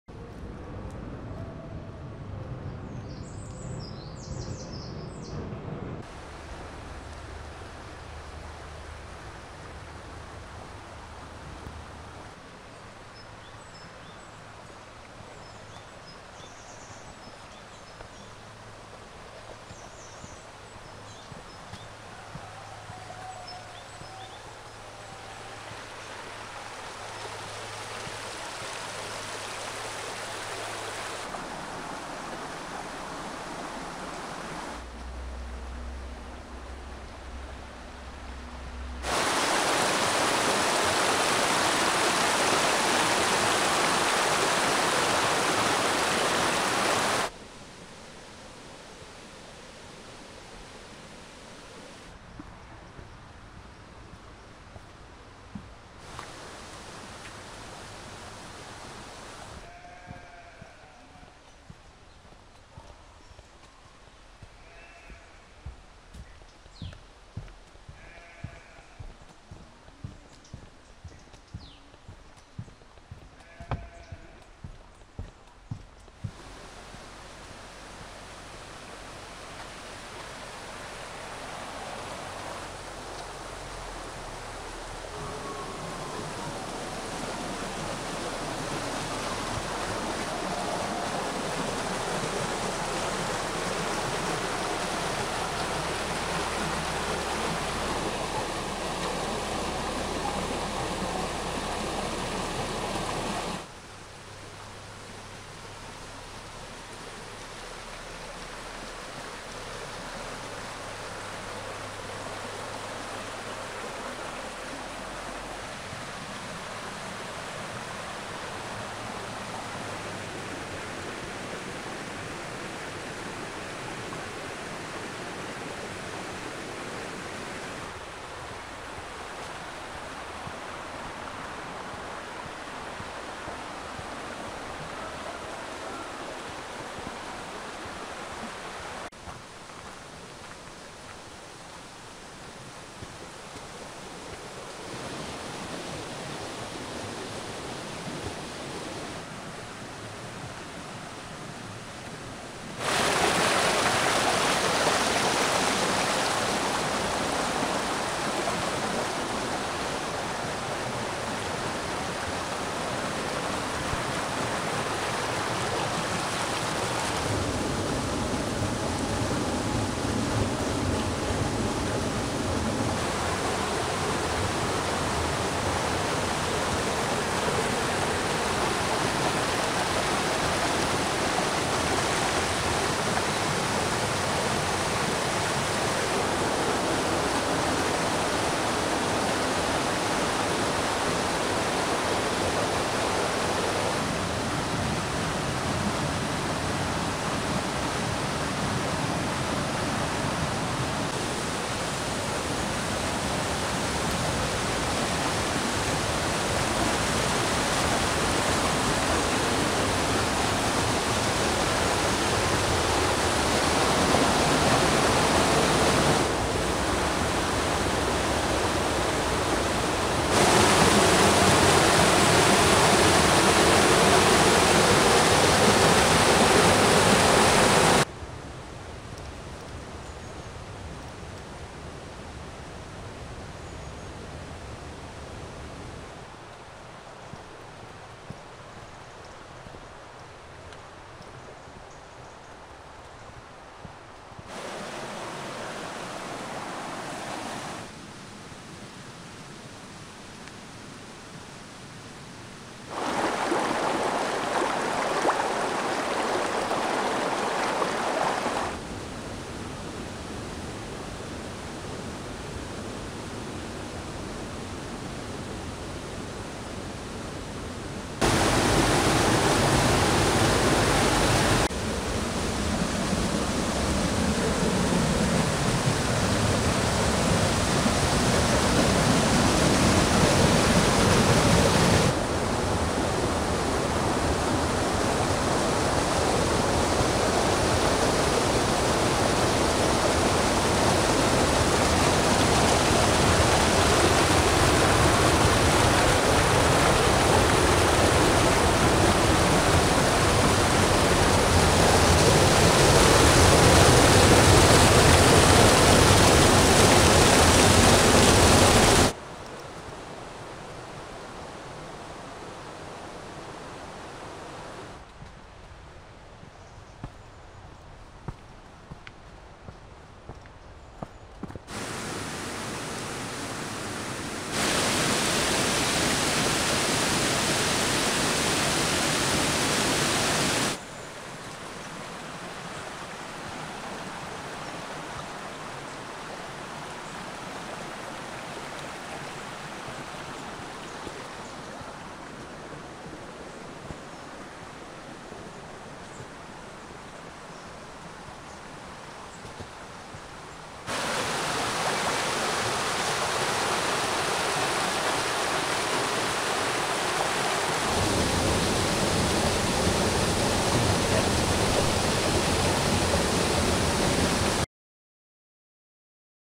Río Majaceite
Es un agradable paseo desde El Bosque a Benamahoma, acompañados porel río Majaceite, que, también es llamado ría de El Bosque y es hábitat de las esquivas nutrias. Descubriremos un excelente bosque de galería; chopos, fresnos y olmos,